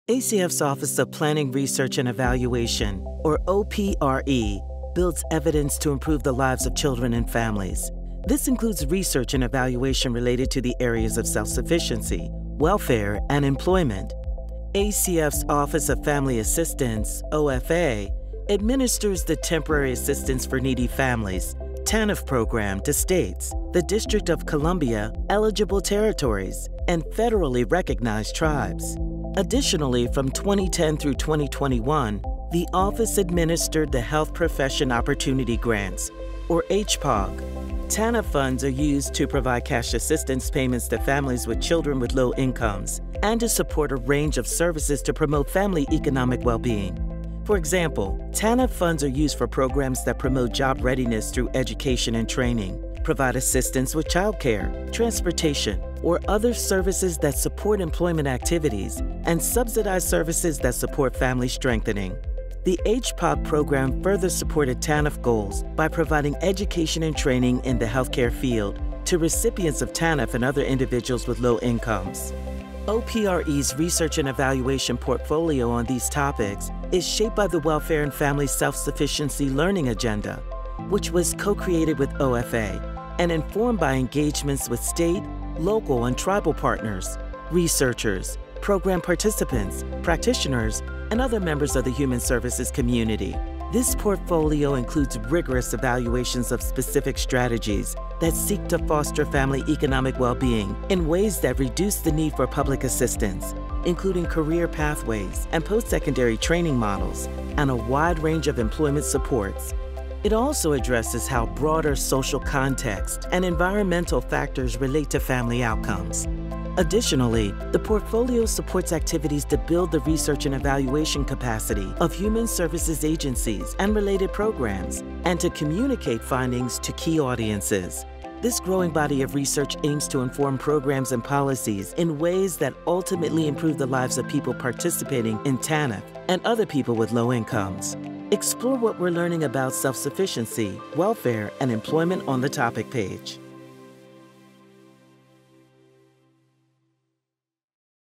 Discovering OPRE's Self-Sufficiency, Welfare, and Employment Portfolio
In this video, discover how OPRE builds evidence to improve the lives of children and families, including through research and evaluation related to the areas of self-sufficiency, welfare, and employment. OPRE’s work supports ACF’s Office of Family Assistance which administers the Temporary Assistance for Needy Families (TANF) program to states, the District of Columbia, eligible territories, and federally recognized tribes. Learn more about OPRE’s efforts to evaluate strategies that seek to foster family economic well-being in ways that reduce the need for public assistance, including career pathways and post-secondary training models and a wide range of employment supports by visiting the Self-Sufficiency, Welfare & Employment topic